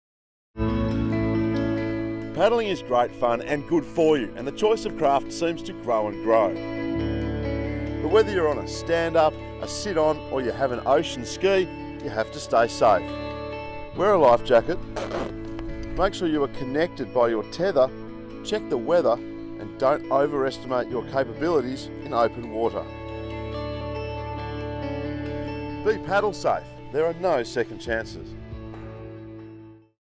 MAST Paddling Safety
Safety reminders for all paddlers – make sure your life jacket is on and make sure you are safely tethered to your craft.
And equally as important, don’t overestimate your ability in your kayak or on your stand-up or sit-on board.